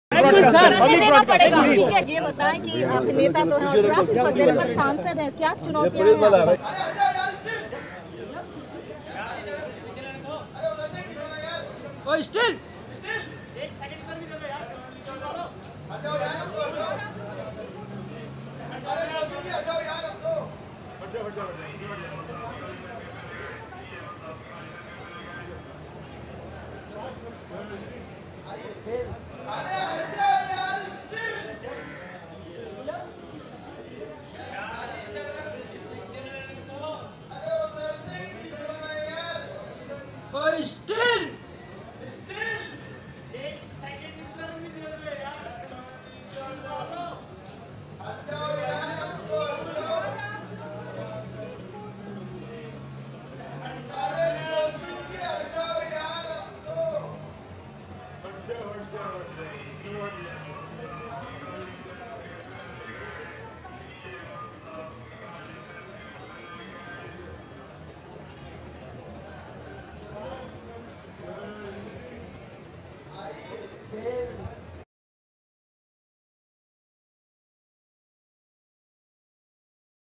Govinda as member of Lok Sabha, stands around with Jaya Prada, Amar Singh why politics and film
Actor Govinda as a member of Lok Sabha, as he stands around with Jaya Prada, Amar Singh. Why do we have this marriage of politics and film?

and